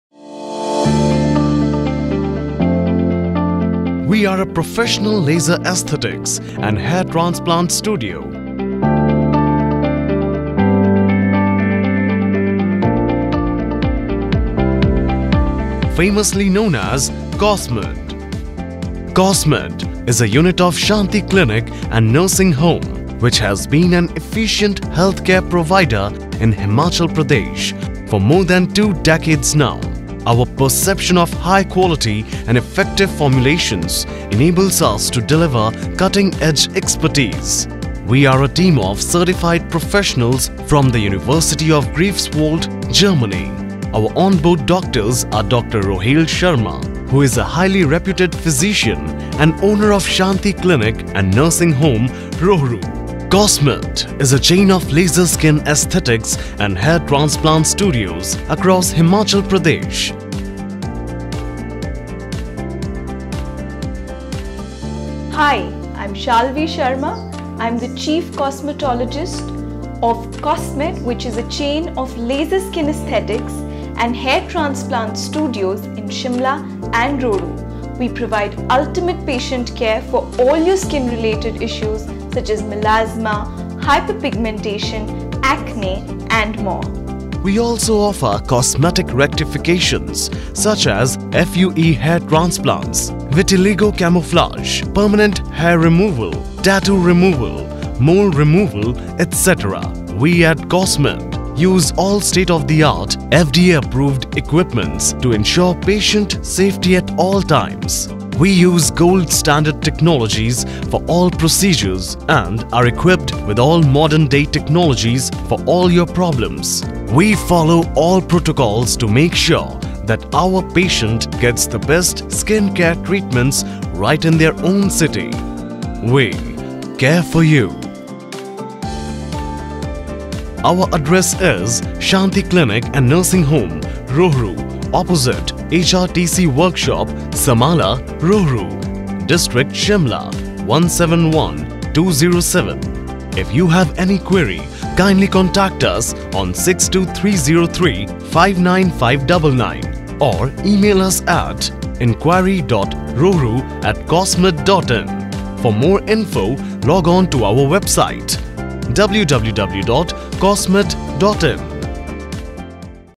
Professional Skin and Hair laser Treatment Studio - Kosmed
Kosmed a pro laser skin aesthetics and hair transplant studio in Shimla and Rohru. Kosmed is a unit of Shanti clinic and nursing home which has been providing effective services for skin problems in Himachal Pradesh. Our motto is to the best treatment, Patient safety using high-quality services and FDA approved equipments with expertise in the newest technology. We are a team of certified professionals including Rohail Sharma, Reputed Psychician, owner of Shanti clinic and Shalvi Sharma chief cosmetologist.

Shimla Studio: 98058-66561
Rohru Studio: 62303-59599